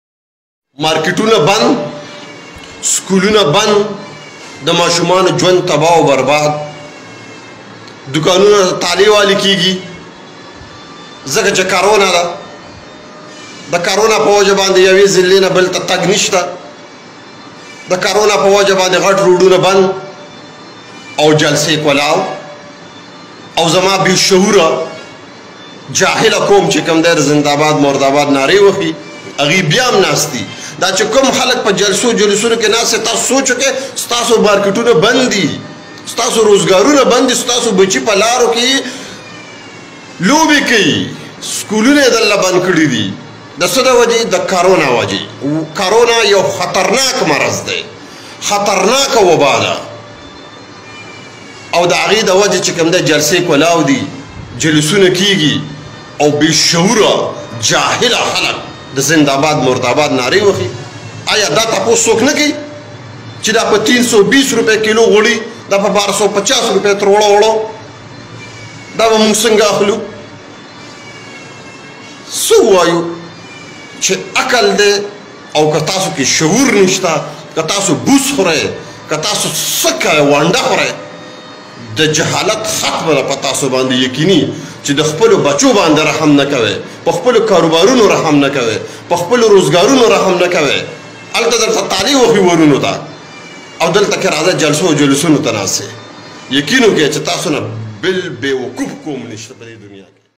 ایک ہی ملک میں دو قانون ۔۔ایک شہری حکومتی پالیسیوں پر برہمسکول بند۔مارکیٹس بند ۔مگر جلسوں کی اجازت
ایک ہی ملک میں دو قانون ۔۔ایک شہری حکومتی پالیسیوں پر برہم
سکول بند۔مارکیٹس بند ۔مگر جلسوں کی اجازت